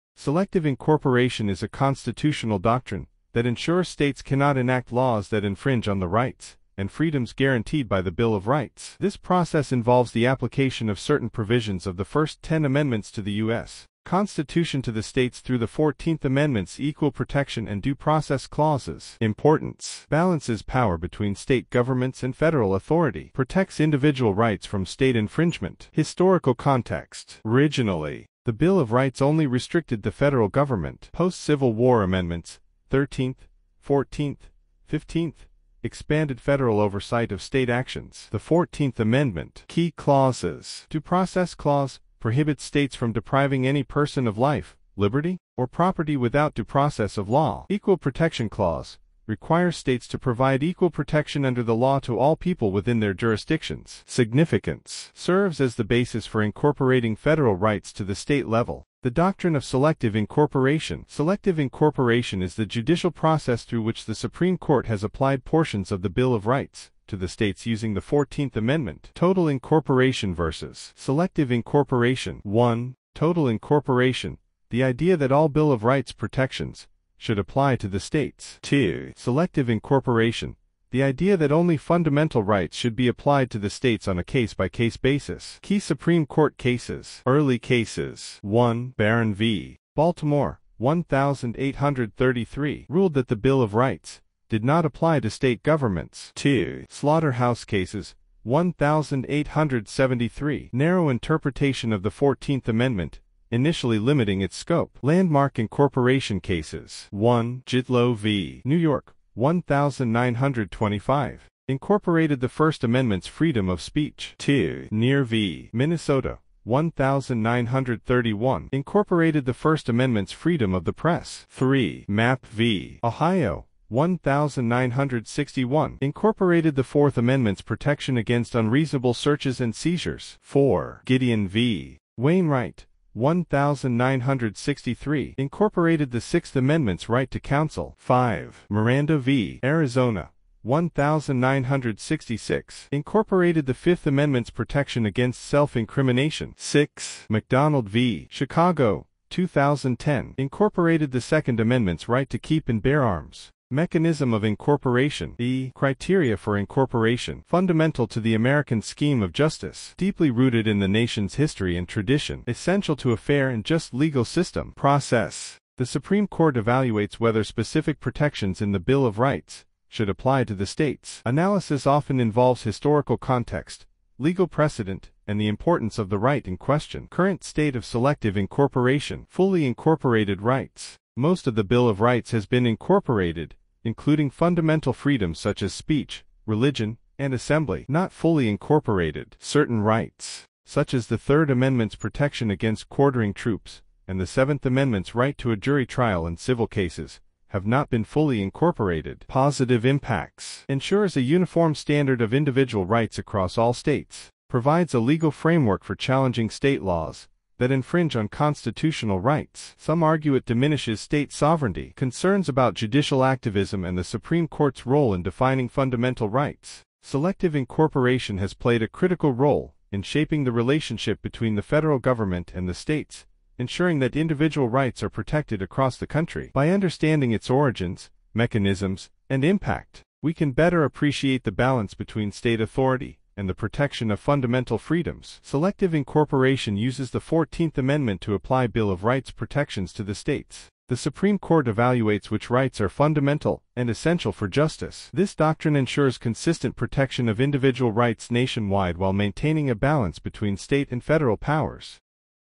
AP Government: Selective Incorporation
Lecture Notes: Selective Incorporation

Introduction
Selective incorporation is a constitutional doctrine that ensures states cannot enact laws that infringe on the rights and freedoms guaranteed by the Bill of Rights. This process involves the application of certain provisions of the first ten amendments to the U.S. Constitution to the states through the Fourteenth Amendment's Equal Protection and Due Process Clauses.

Importance:
- Balances power between state governments and federal authority.
- Protects individual rights from state infringement.

Historical Context:
- Originally, the Bill of Rights only restricted the federal government.
- Post-Civil War amendments (13th, 14th, 15th) expanded federal oversight of state actions.
The Fourteenth Amendment
Key Clauses:
1. Due Process Clause: Prohibits states from depriving any person of life, liberty, or property without due process of law.
2. Equal Protection Clause: Requires states to provide equal protection under the law to all people within their jurisdictions.

Significance:
- Serves as the basis for incorporating federal rights to the state level.

The Doctrine of Selective Incorporation
Definition:
Selective incorporation is the judicial process through which the Supreme Court has applied portions of the Bill of Rights to the states using the Fourteenth Amendment.

Total Incorporation vs. Selective Incorporation:
1. Total Incorporation: The idea that all Bill of Rights protections should apply to the states.
2. Selective Incorporation: The idea that only fundamental rights should be applied to the states on a case-by-case basis.

Key Supreme Court Cases
Early Cases:
1. Barron v. Baltimore (1833)
   - Ruled that the Bill of Rights did not apply to state governments.
2. Slaughter-House Cases (1873)
   - Narrow interpretation of the Fourteenth Amendment, initially limiting its scope.

Landmark Incorporation Cases:
1. Gitlow v. New York (1925)
   - Incorporated the First Amendment's freedom of speech.
2. Near v. Minnesota (1931)
   - Incorporated the First Amendment's freedom of the press.
3. Mapp v. Ohio (1961)
   - Incorporated the Fourth Amendment's protection against unreasonable searches and seizures.
4. Gideon v. Wainwright (1963)
   - Incorporated the Sixth Amendment's right to counsel.
5. Miranda v. Arizona (1966)
   - Incorporated the Fifth Amendment's protection against self-incrimination.
6. McDonald v. Chicago (2010)
   - Incorporated the Second Amendment's right to keep and bear arms.

Mechanism of Incorporation
Criteria for Incorporation:
- Fundamental to the American scheme of justice.
- Deeply rooted in the nation's history and tradition.
- Essential to a fair and just legal system.

Process:
- The Supreme Court evaluates whether specific protections in the Bill of Rights should apply to the states.
- Analysis often involves historical context, legal precedent, and the importance of the right in question.

Current State of Selective Incorporation
Fully Incorporated Rights:
- Most of the Bill of Rights has been incorporated, including fundamental freedoms such as speech, religion, and assembly.

Not Fully Incorporated:
- Certain rights, such as the Third Amendment's protection against quartering troops and the Seventh Amendment's right to a jury trial in civil cases, have not been fully incorporated.

Implications and Criticisms
Positive Impacts:
- Ensures a uniform standard of individual rights across all states.
- Provides a legal framework for challenging state laws that infringe on constitutional rights.

Criticisms:
- Some argue it diminishes state sovereignty.
- Concerns about judicial activism and the Supreme Court's role in defining fundamental rights.

Conclusion
Selective incorporation has played a critical role in shaping the relationship between the federal government and the states, ensuring that individual rights are protected across the country. By understanding its origins, mechanisms, and impact, we can better appreciate the balance between state authority and the protection of fundamental freedoms.

Key Takeaways:
- Selective incorporation uses the Fourteenth Amendment to apply Bill of Rights protections to the states.
- The Supreme Court evaluates which rights are fundamental and essential for justice.
- This doctrine ensures consistent protection of individual rights nationwide while maintaining a balance between state and federal powers.